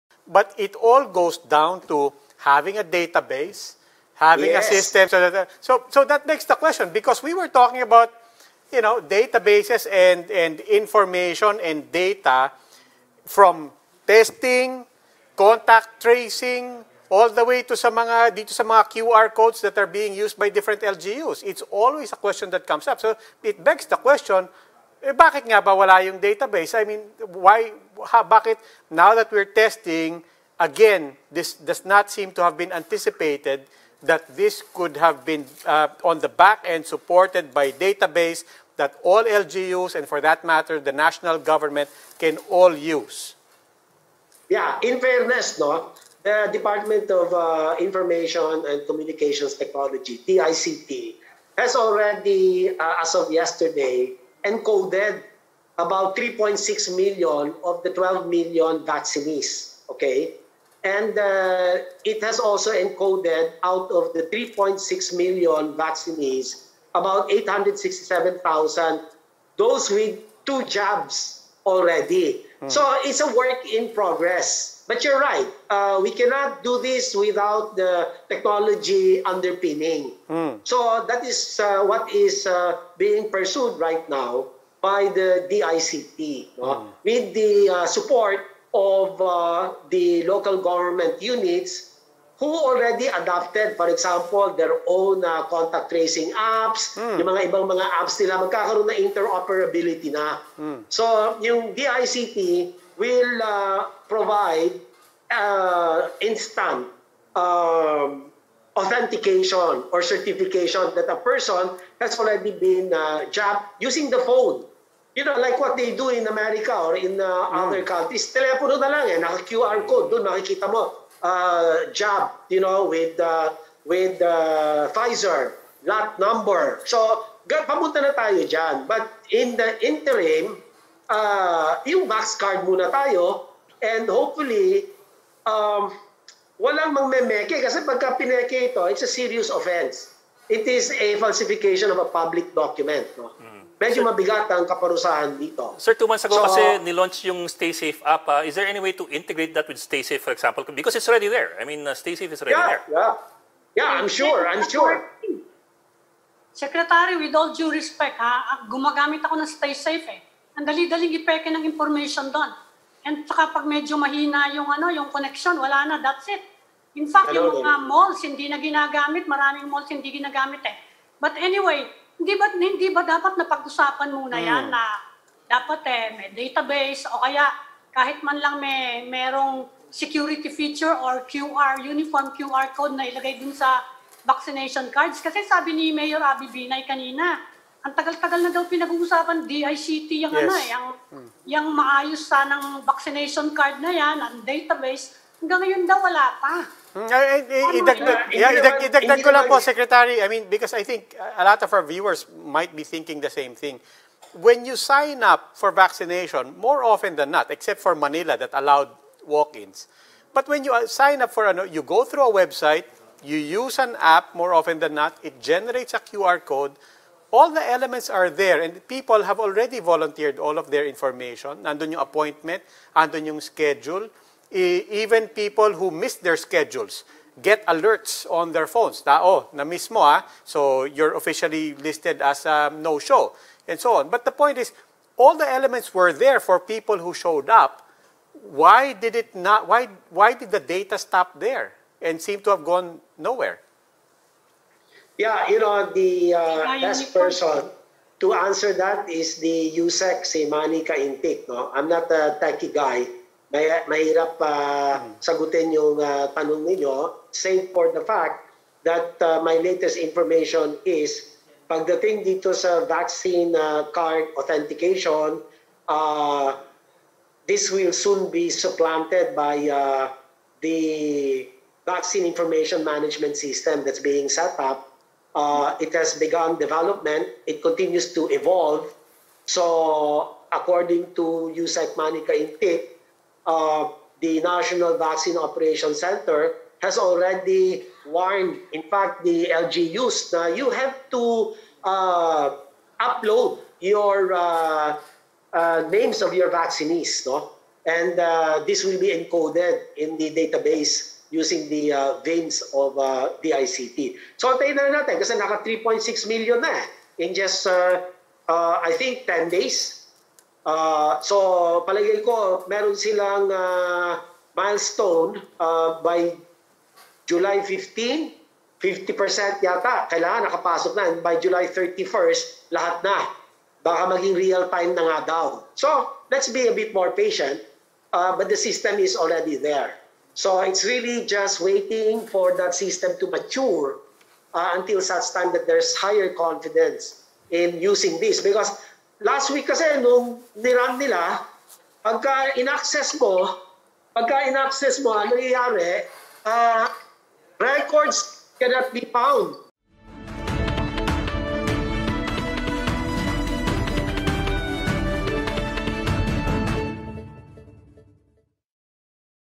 DOH working with DICT to set up vaccination verification through phone
Sec. Francisco Duque III says the Department of Health is currently working with the Department of Information and Communications Technology to set up a vaccination verfication system by phone after some concerns from LGUs on fake vaccination cards.